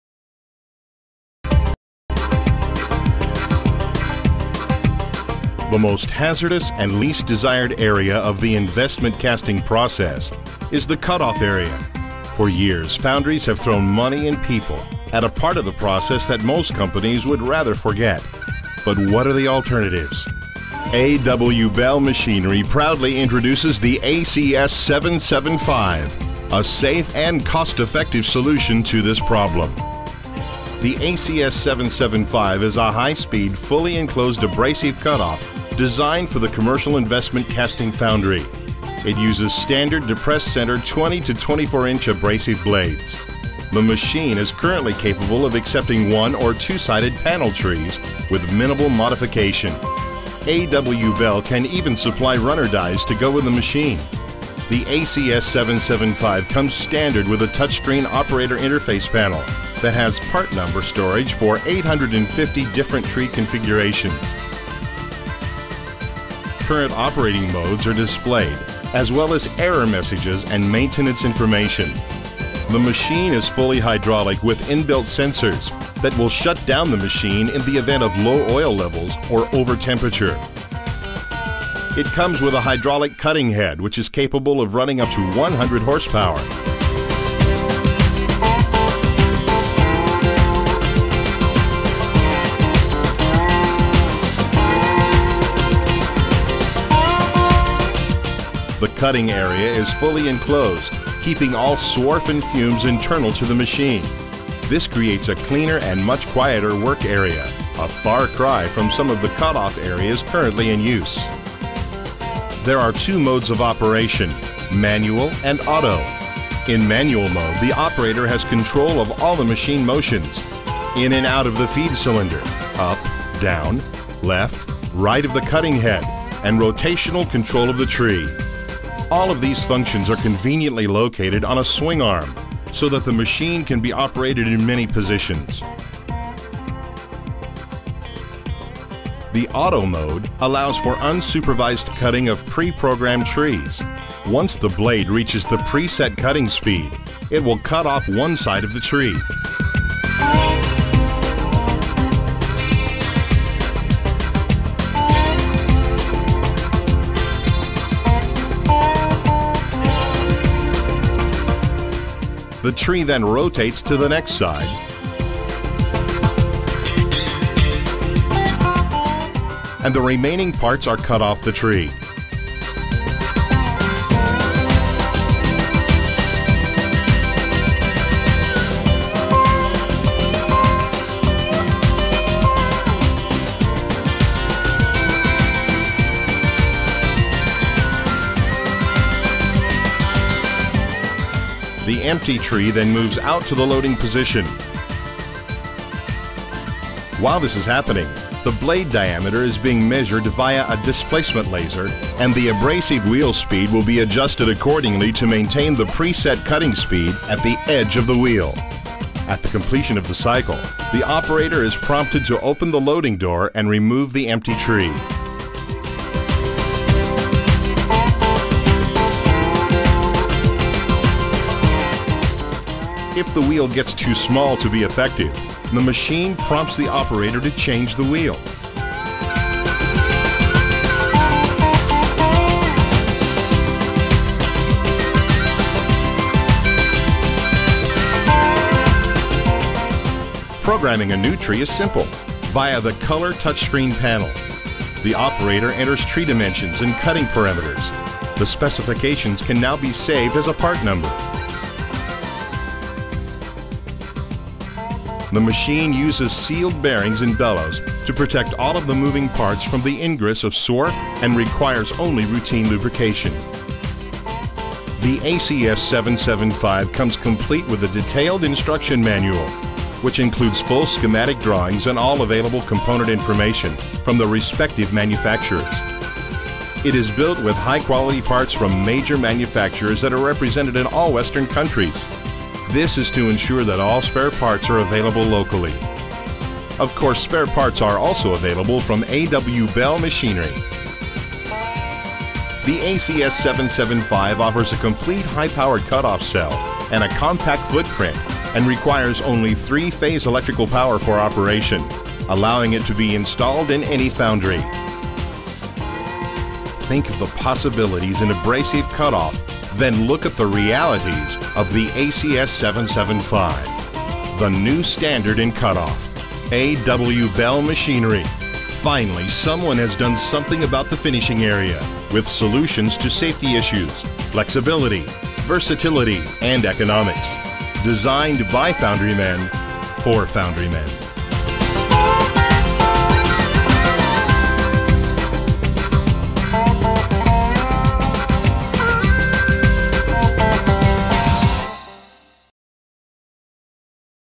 ACS 775 Programmable Abrasive Cutoff Machine
A fully programmable abrasive wheel cut-off machine that utilises a 20"- 24" (500mm-600mm) abrasive wheel. The ACS 775 is designed to use standard 3-5 bar flat runner or square investment casting trees with minimal modifications.

A.W.Bell Machinery
145 Abbotts Road
Dandenong South 3175
Australia